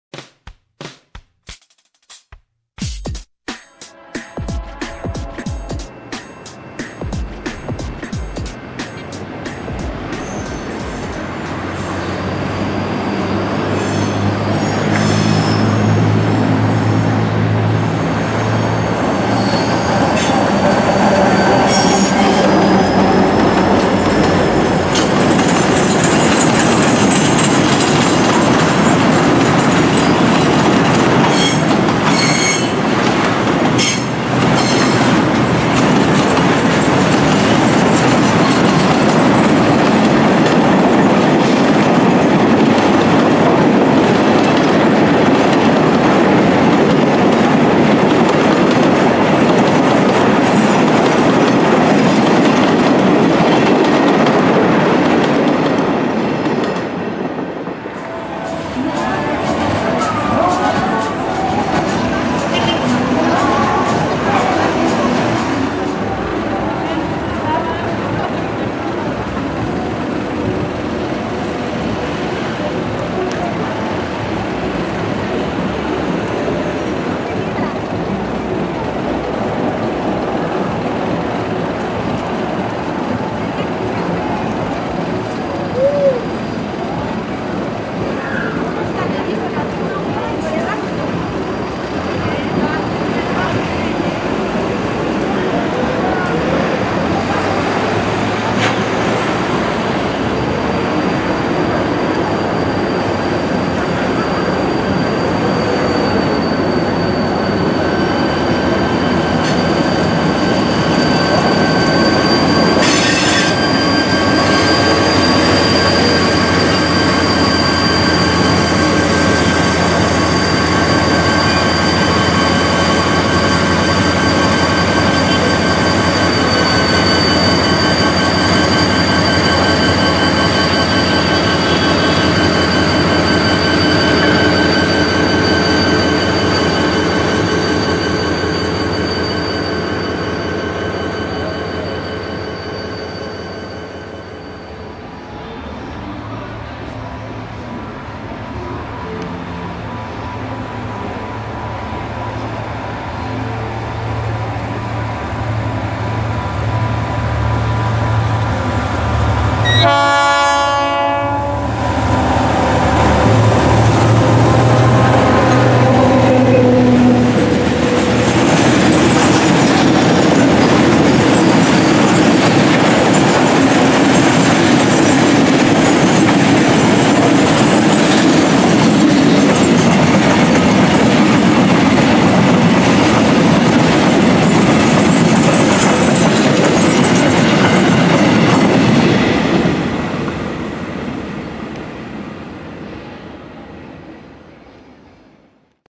FEPASA | PORTALES / BELLAVISTA / QUILPUÉ
Tren bajando al puerto cargado con cobre filmado en Portales, Bellavista, para luego ser cargado de Contenedores ya de vuelta a Santiago, filmado en Quilpué.

METROTRENHD16
CAZATRENES2016